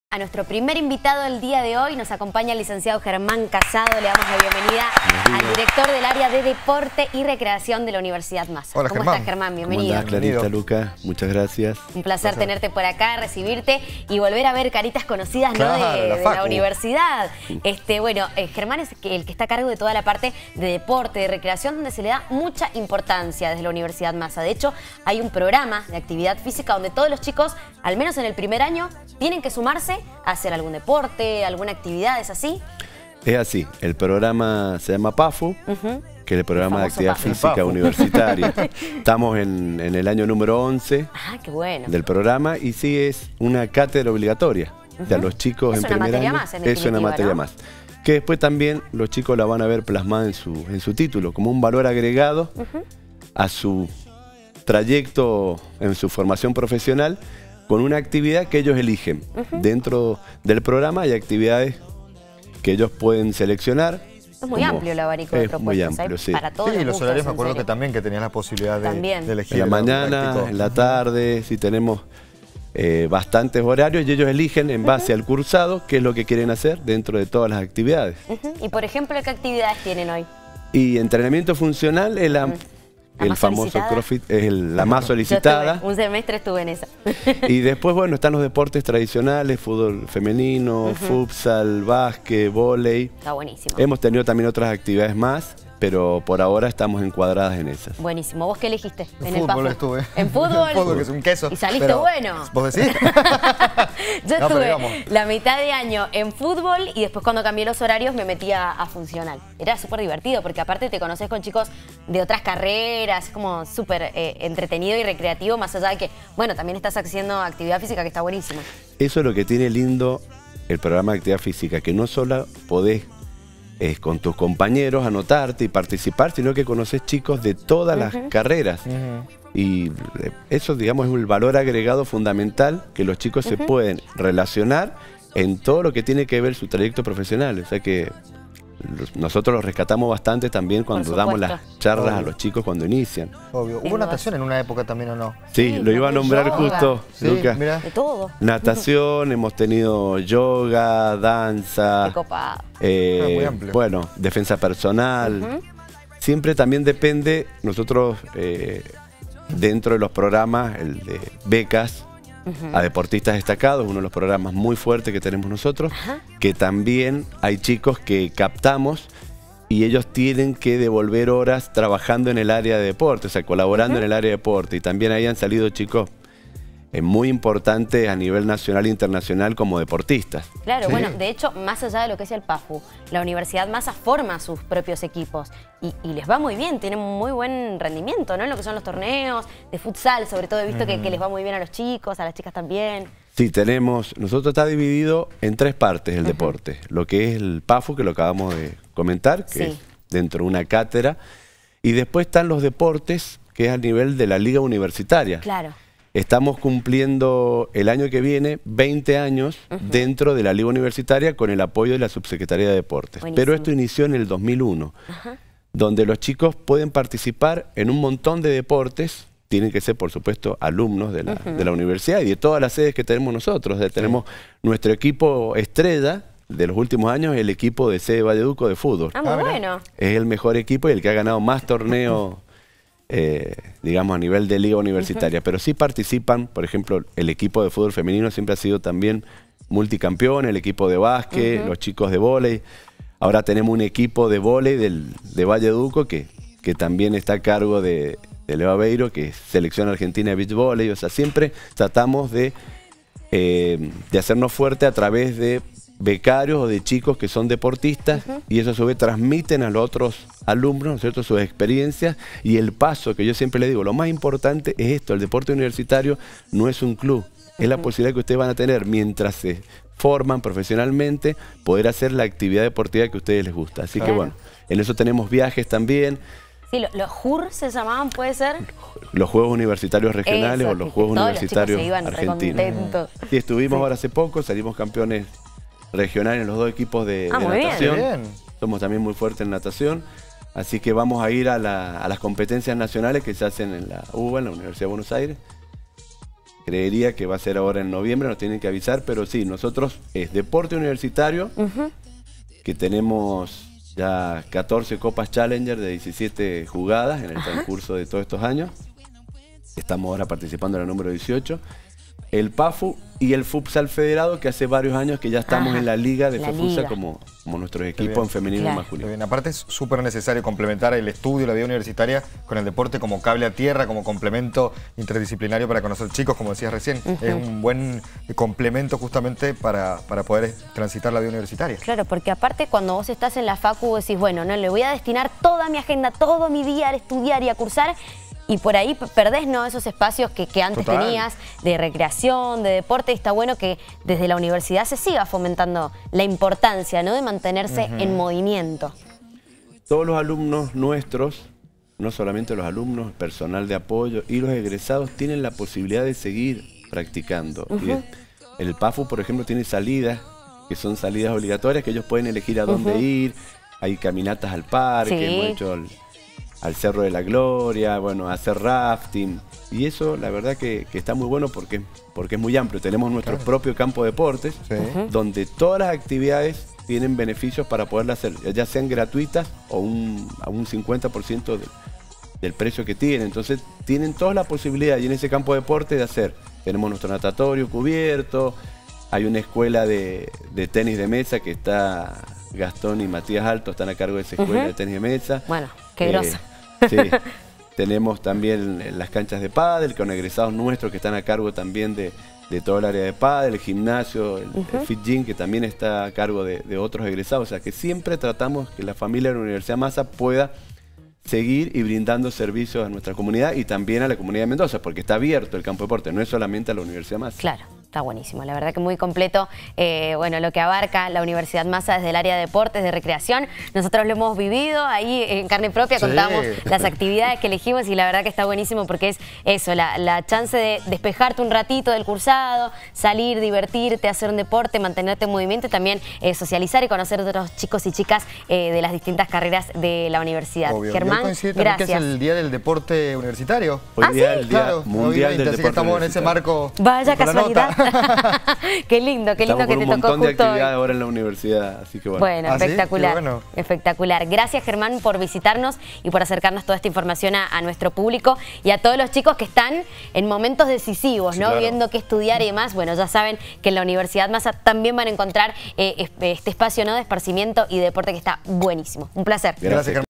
UNIVERSIDAD MAZA | El deporte como valor agregado en su formación profesional
Licenciado Germán Casado Director de Deporte y Recreación de la Universidad Maza nos visitó para hablar la importancia de la actividad física como acompañamiento del crecimiento de los alumnos en el desarrollo de su carrera universitaria; es por esto que los alumnos de todas las carreras de esta institución tienen acceso a distintos deportes que ofrece la universidad.